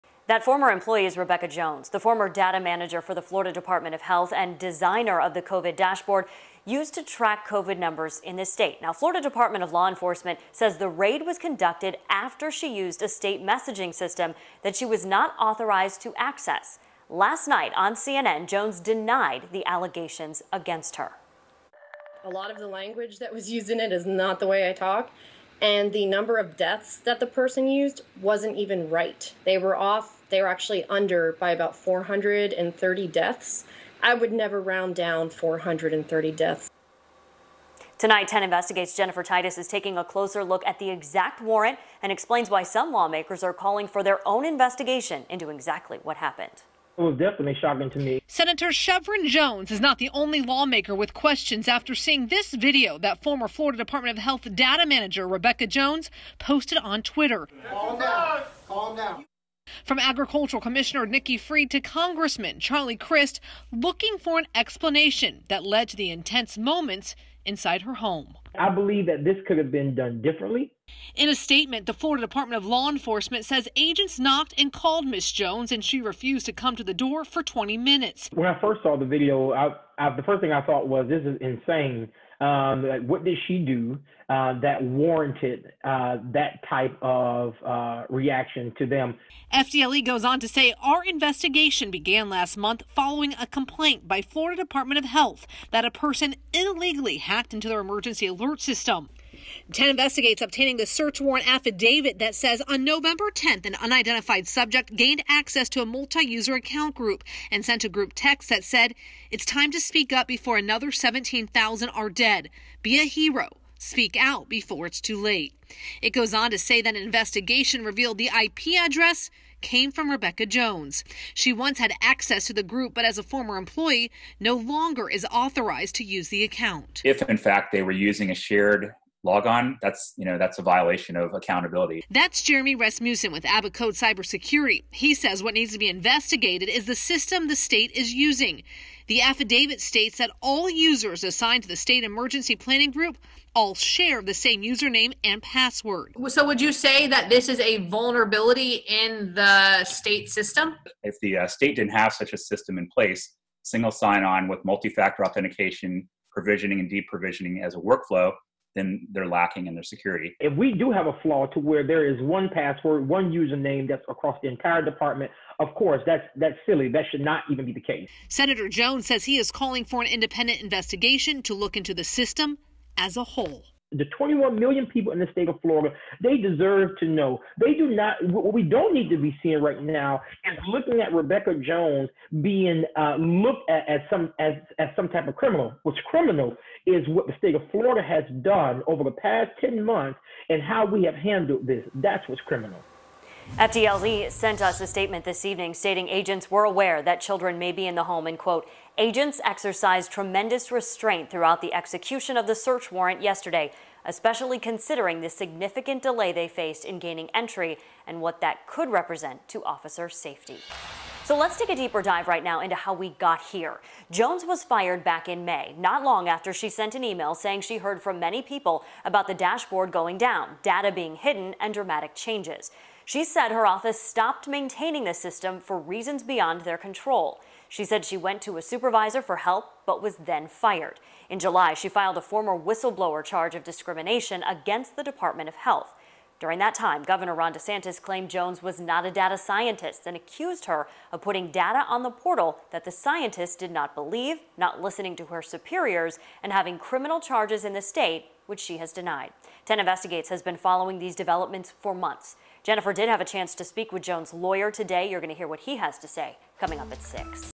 State leaders speak out after former DOH employee's home is raided
Sen. Shevrin Jones wants an investigation into the raid at the home of Rebekah Jones.